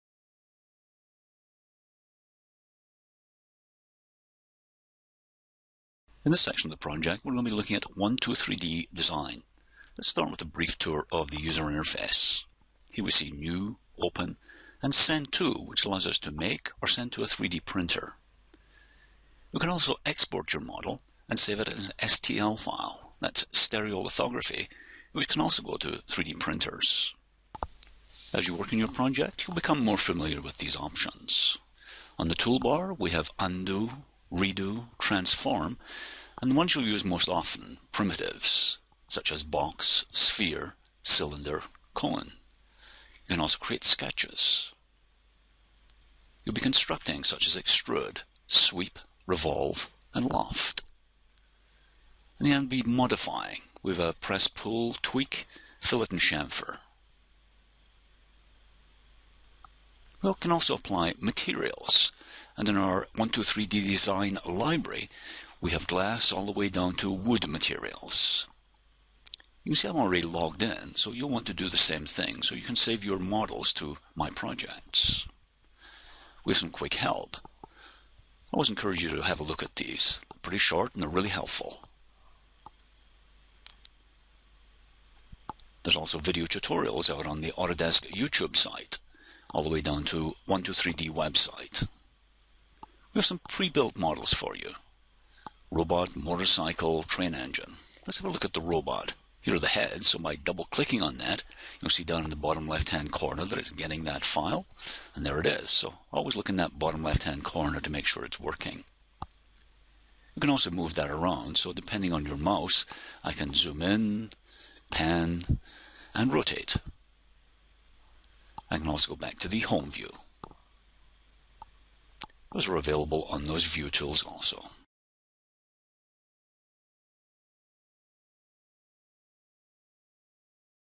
Introduction to Navigation in Autodesk 123D Design
An introduction to the Autodesk 123D Design user interface/navigation.